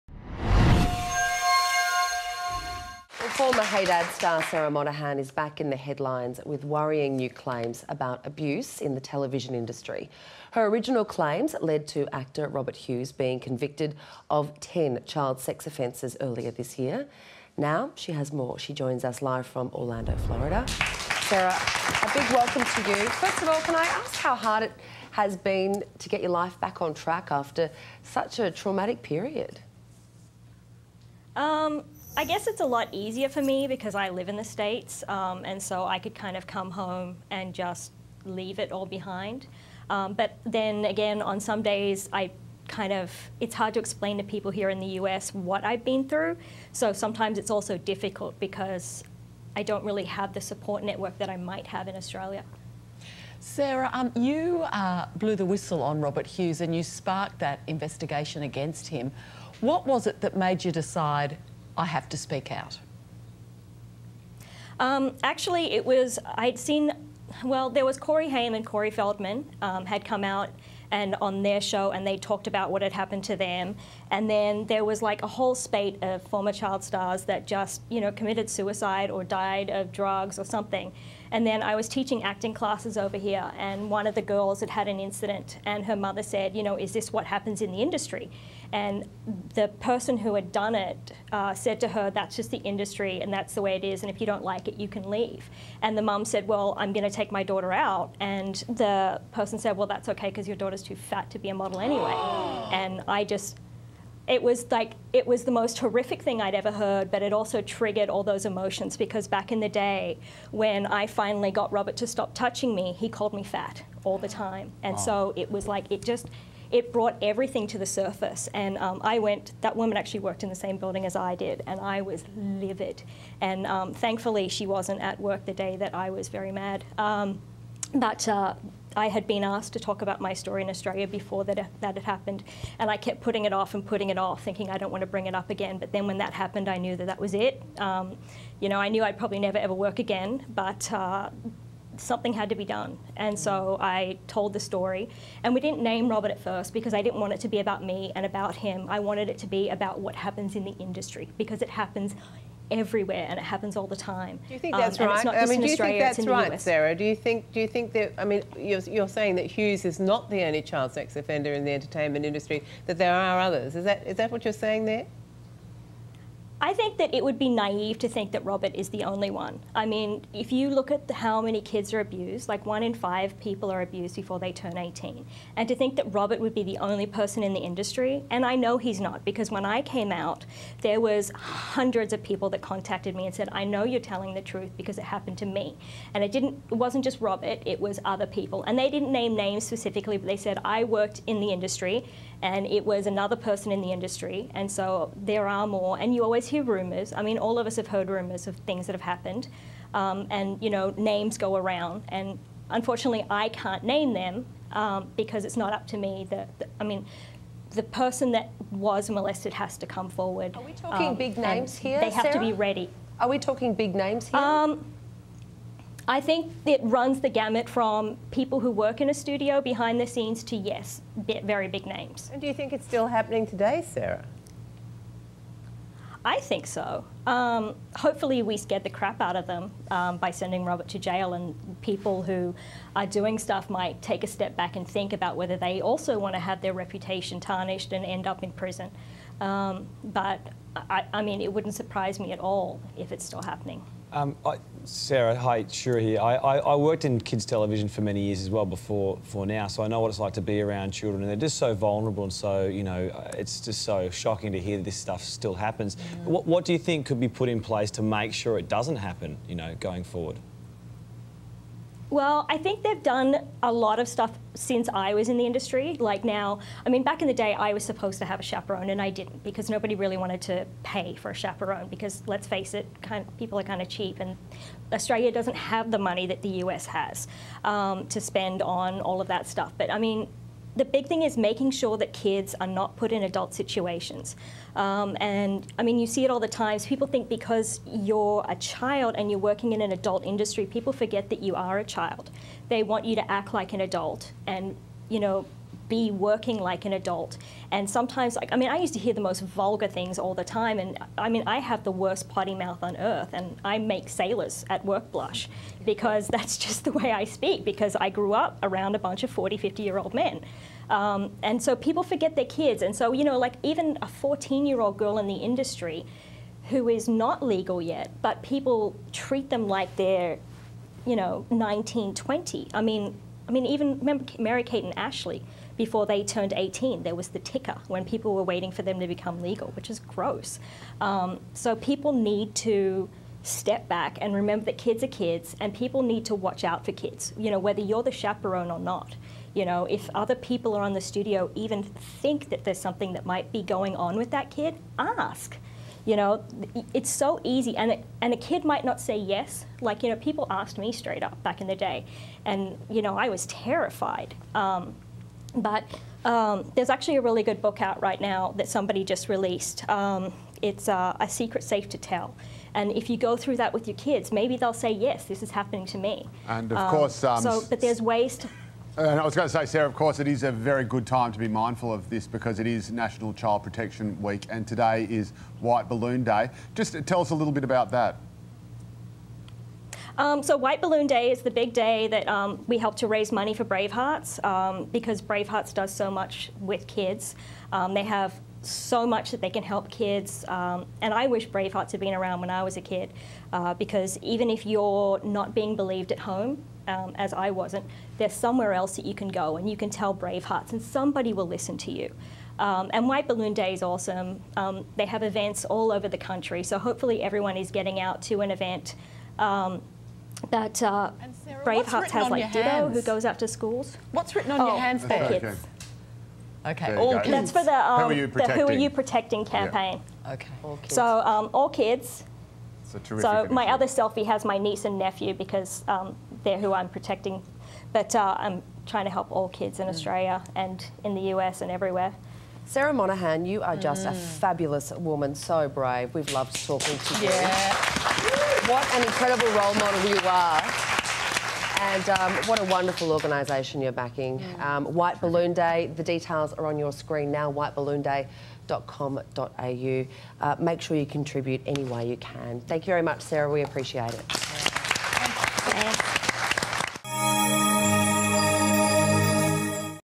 Sarah Monahan: Speaking Out About Abuse
Former Hey Dad star Sarah Monahan is back in the headlines with worrying new claims about abuse in the TV industry.